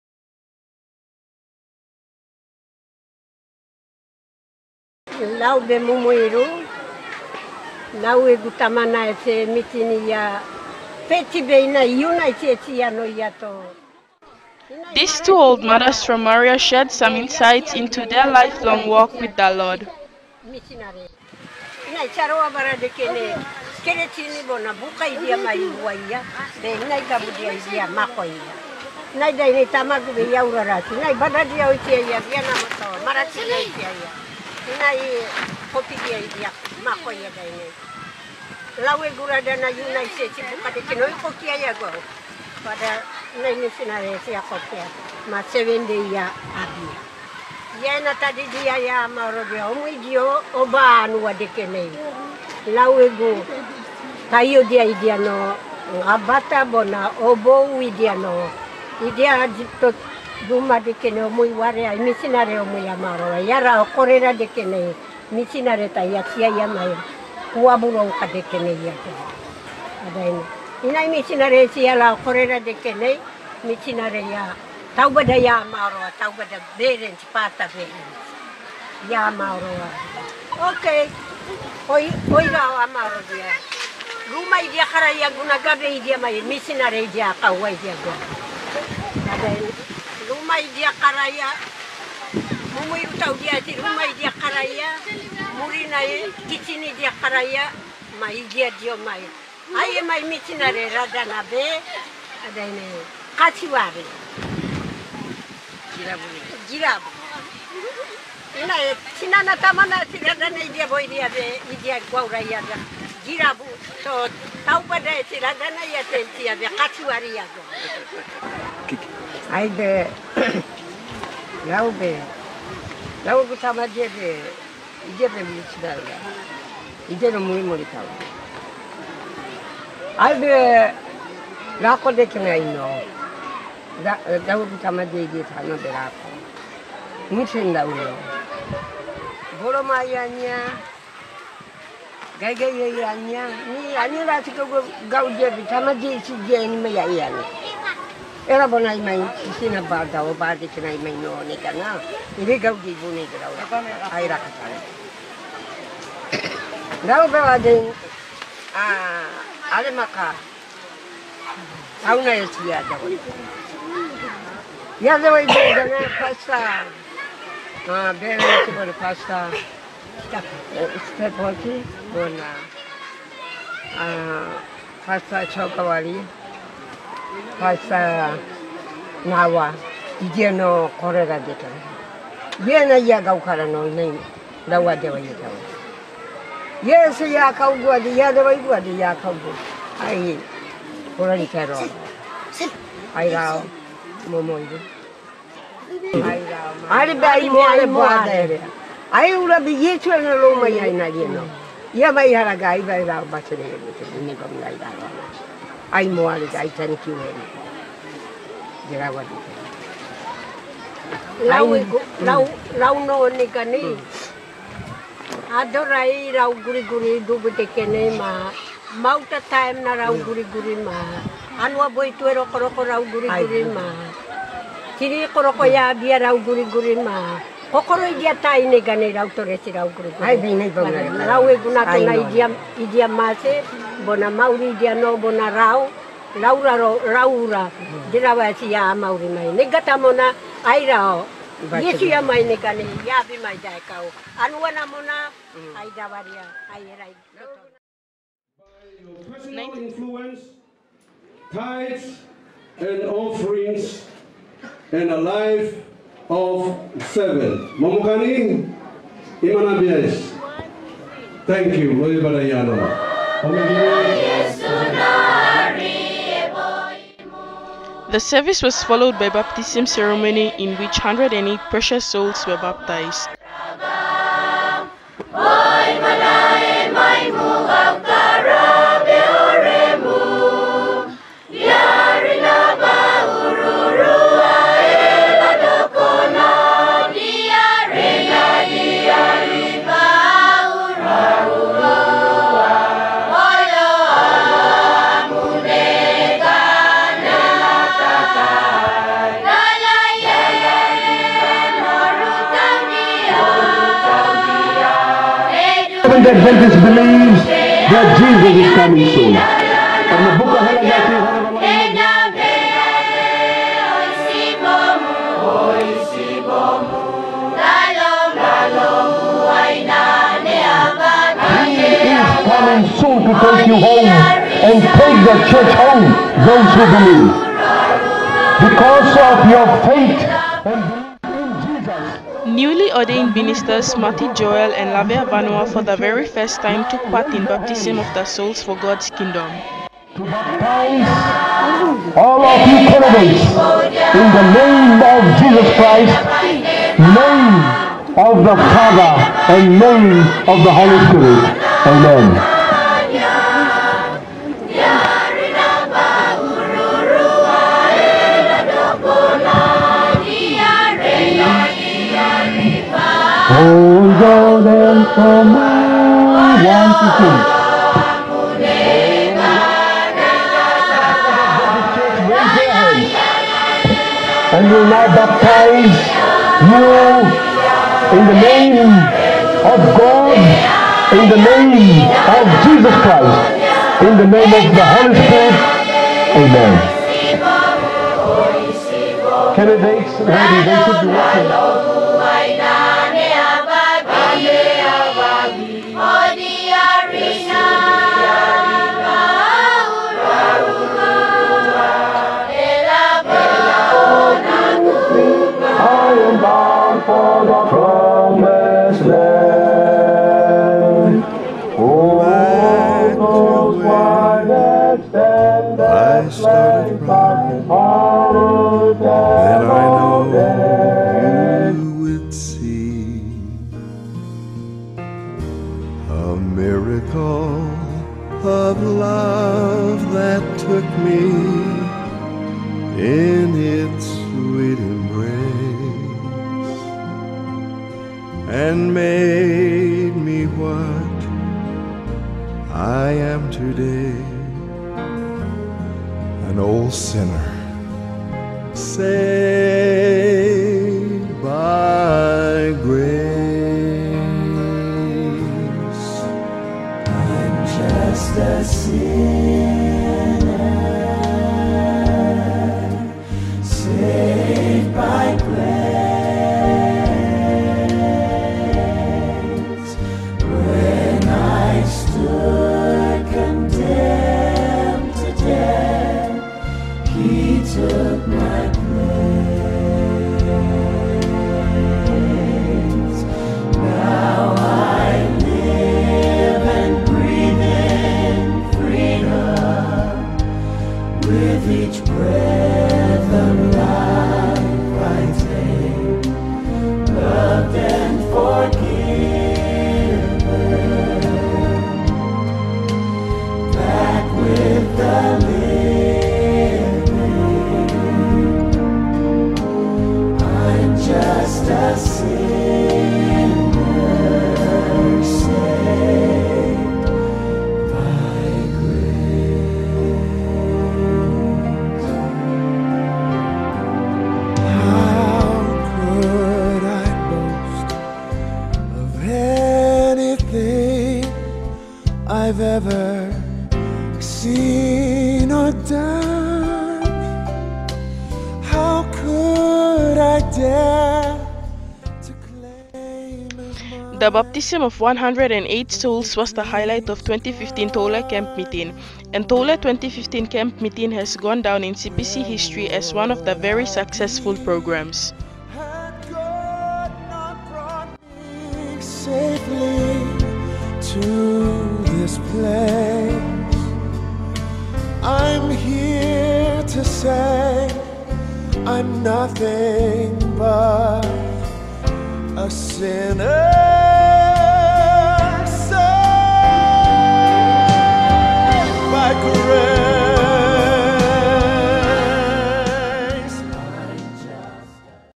TOULE CPC 2015 CAMP MEETING MEMORIES (BAPTISM AND 2 OLD FAITHFUL MOTHERS INTERVIEW)
CHURCH PROGRAM